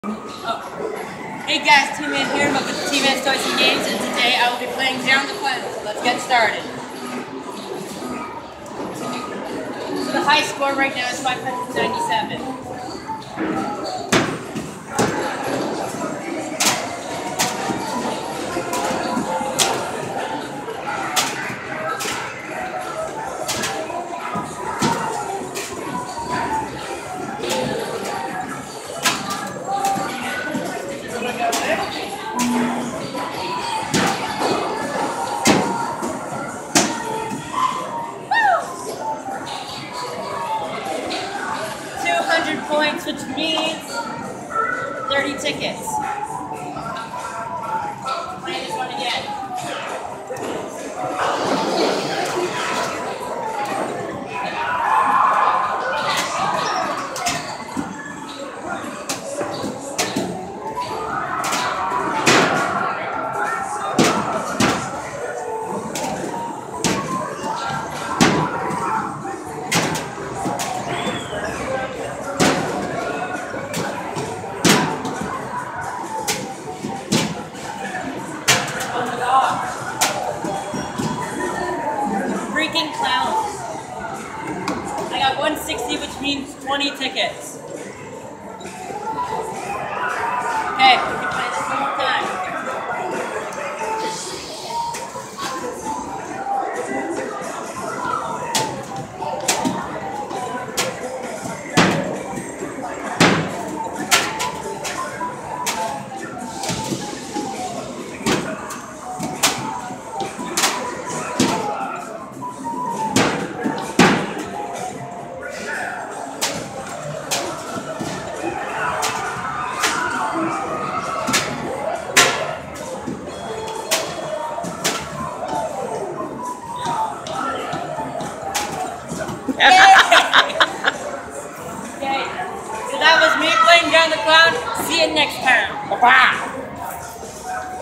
Playing Down the Clown at our local Arcade
Taking aim and knocking out those clowns to win tickets for prizes.  Trying for the Jackpot win.